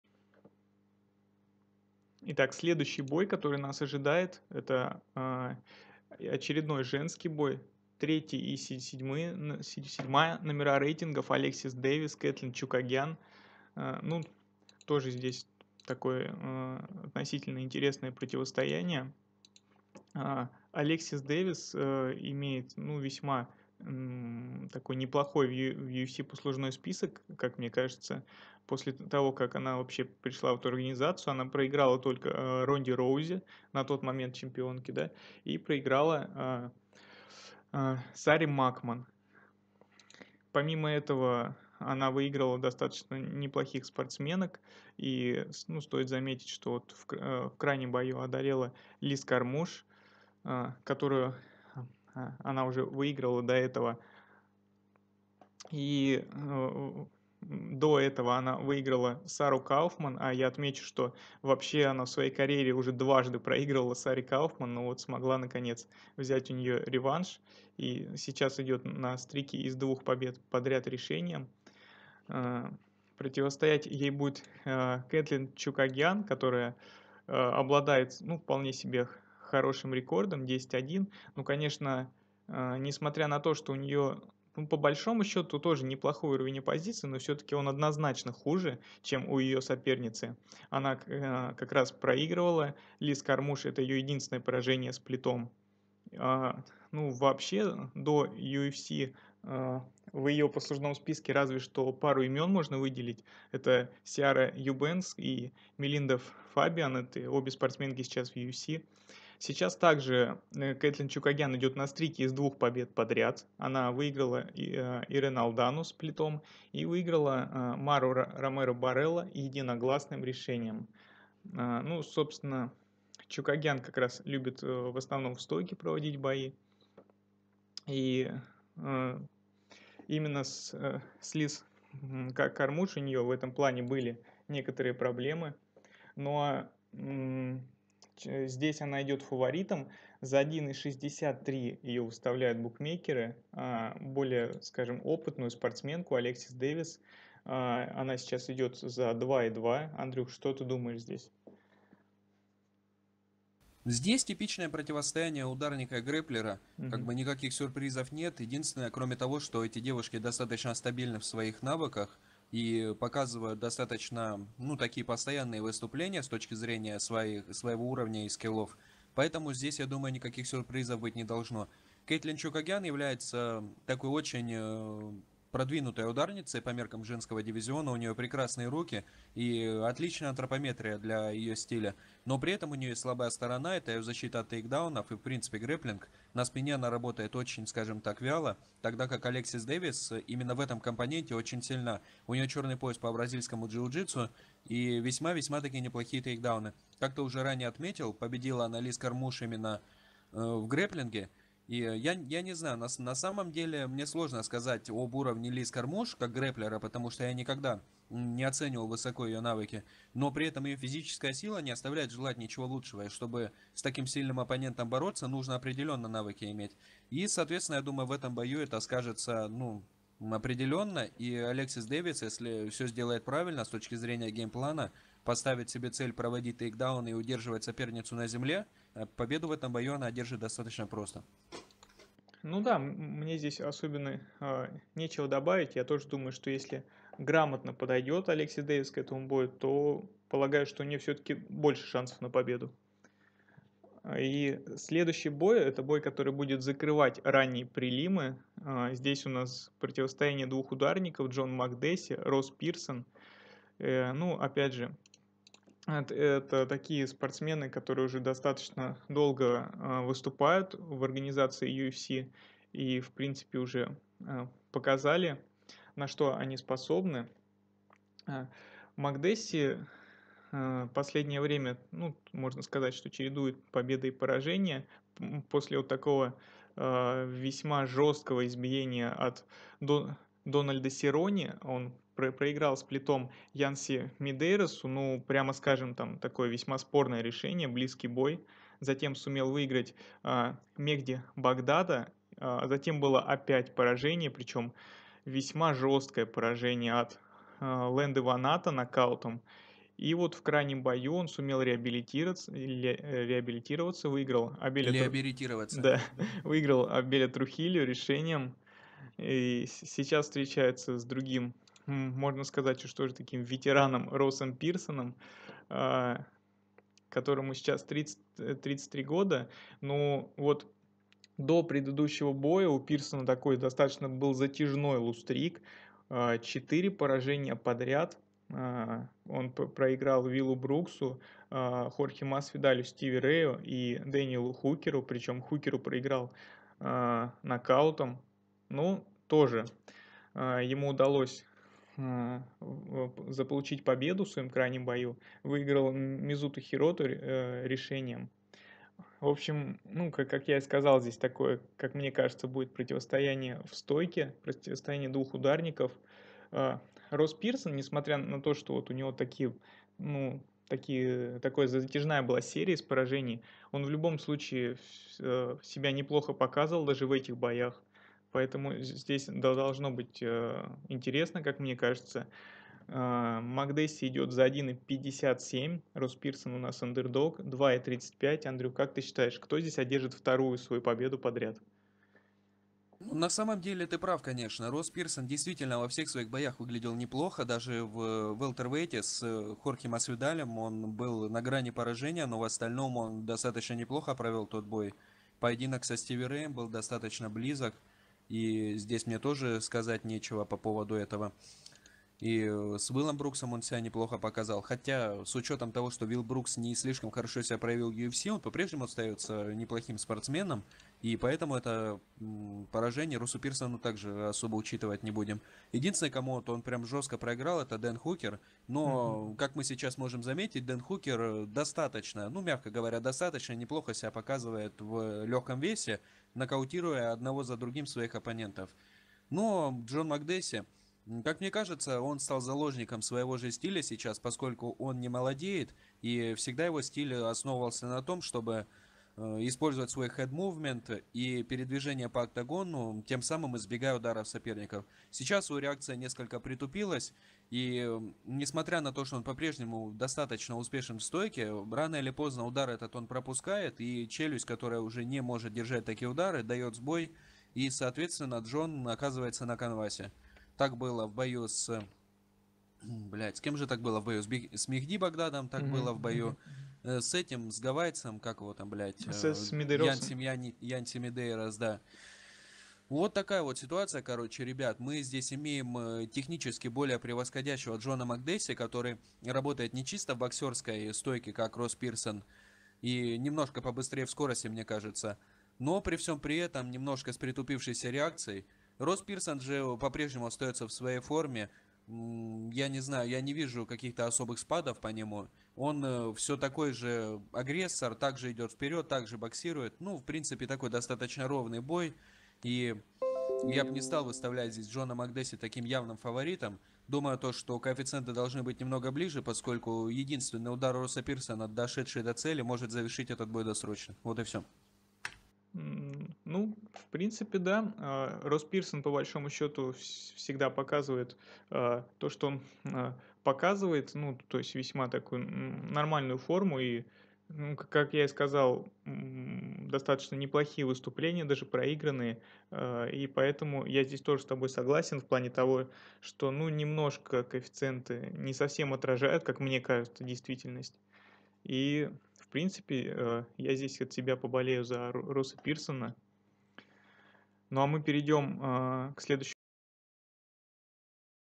Прогноз и аналитика от MMABets UFC on FOX 30: Макдесси Пирсон, Дэвис-Чукагян. Выпуск №107. Часть 2/6
Прогноз на бои турнира UFC on FOX 30 от команды MMABets. Часть 2

Если у кого то появилось желание отблагодарить за подкаст и бесплатные ставки, то это можно сделать по следующим реквизитам:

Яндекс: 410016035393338